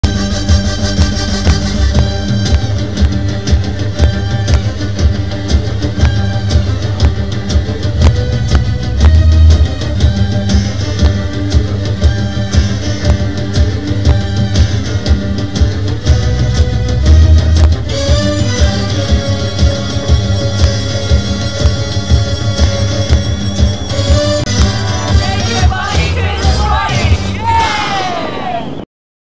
Sydney Harbour Ferry Cruise
My sailing Club provides a Spectator Ferry during the sailing season,most saturdays. One of Sydneys` Bargain Water Treats  !!